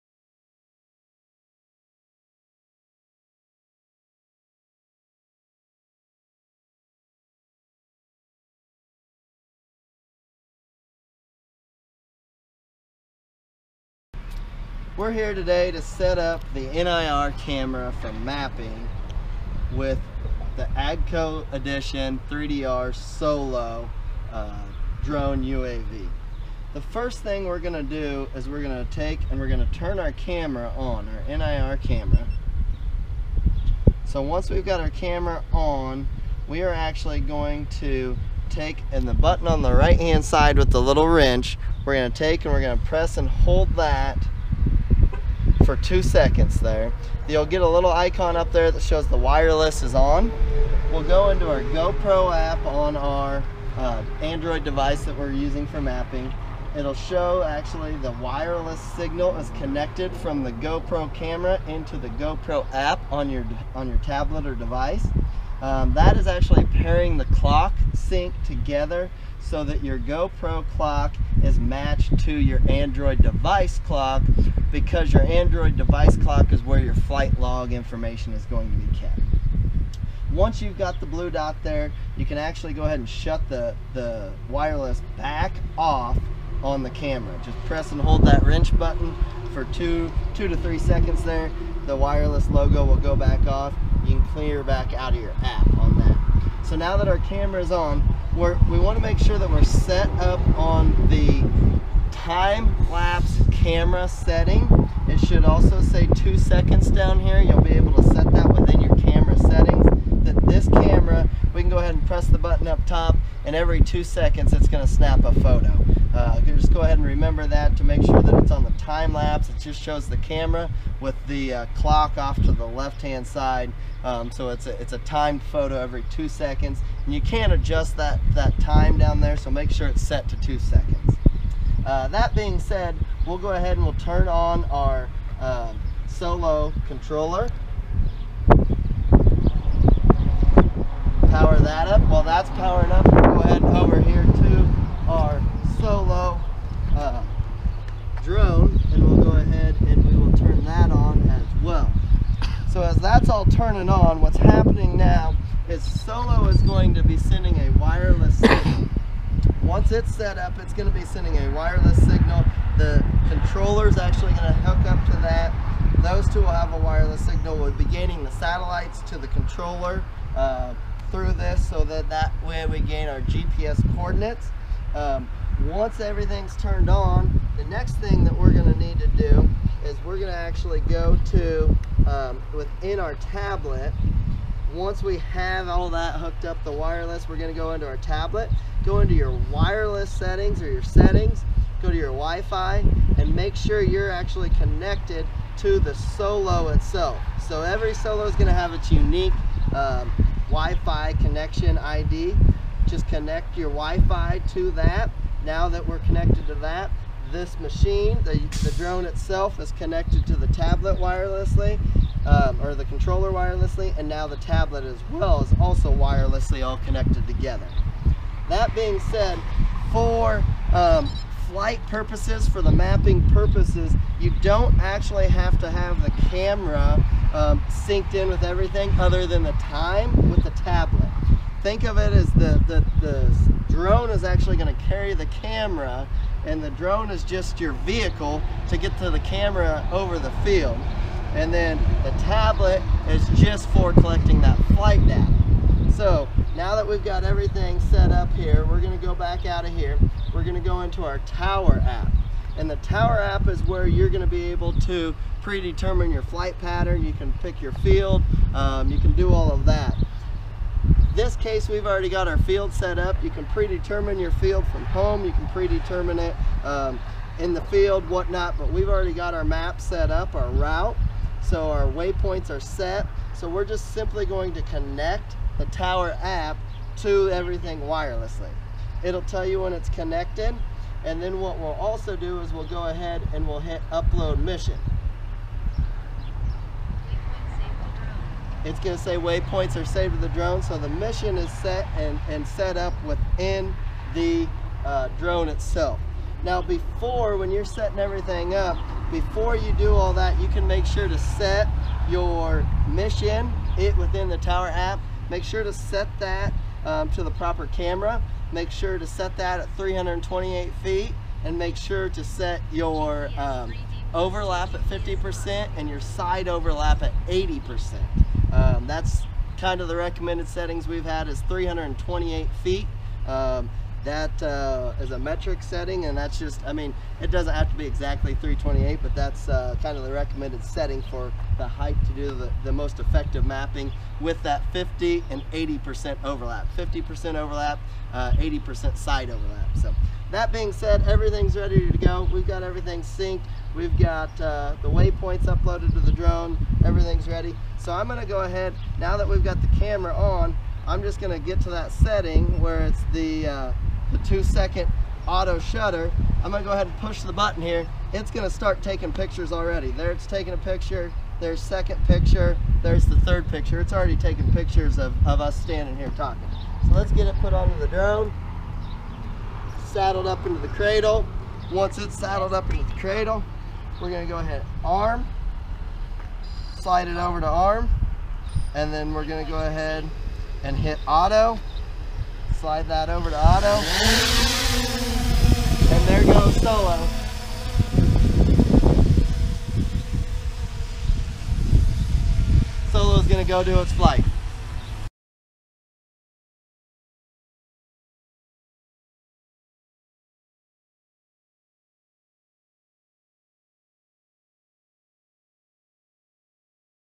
AGCO SOLO - Setting Up NIR Camera & SOLO for Mapping
AGCO SOLO is a UAV/Drone used for crop health mapping, NDVI.

This video is to help the user make sure everything is set up properly.

Make sure to take your time and follow each step.  Every little detail needs to be right to make sure you get the proper results.  Do not hurry through, take your time and focus on the task at hand!